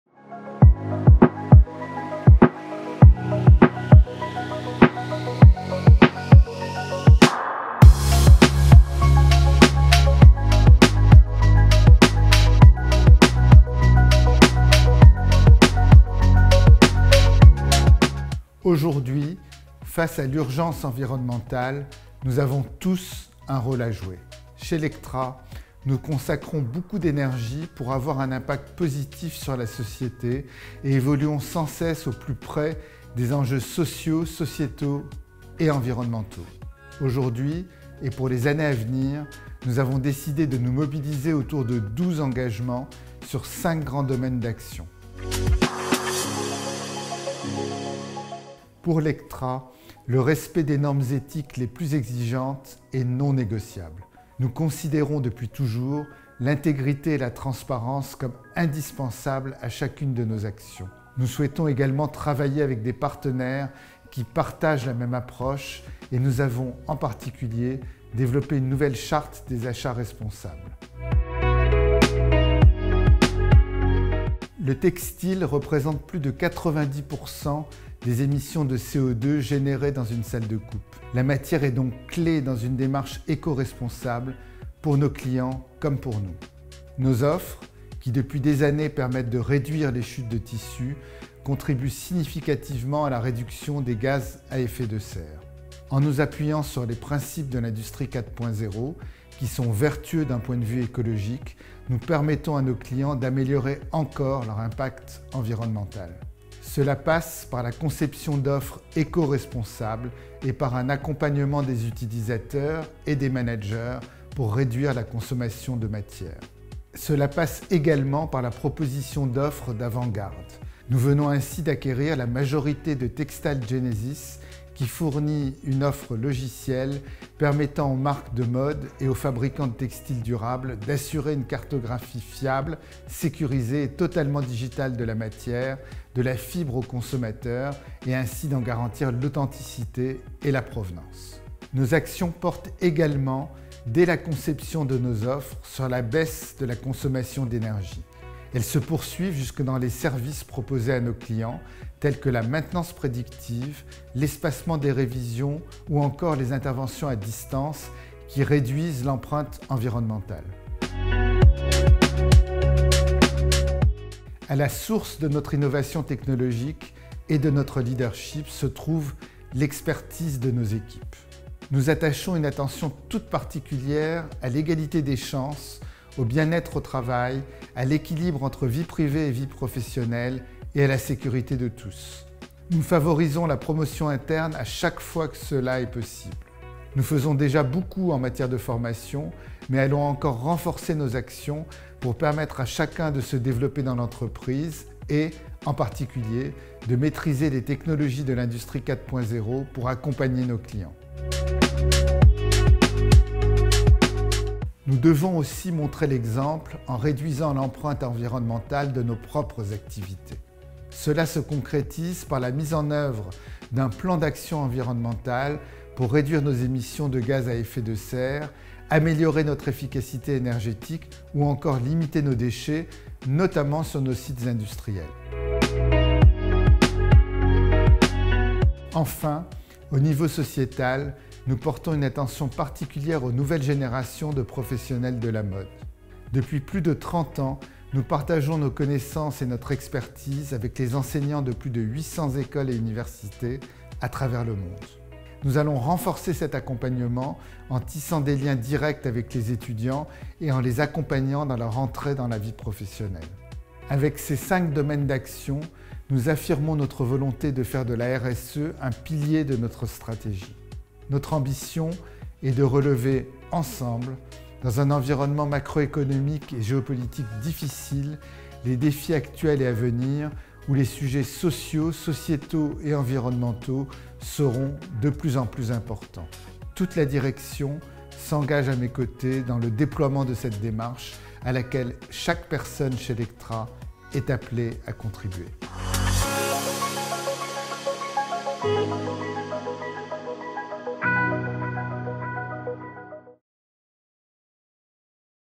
ITW Daniel Harari- La Responsabilité Sociale des Entreprises (RSE) chez Lectra
We are sustainability enablers
Nous avons aussi compris depuis longtemps que la responsabilité sociétale devait faire partie intégrante de notre stratégie. Mais aujourd’hui, nous avons décidé d’aller encore plus loin.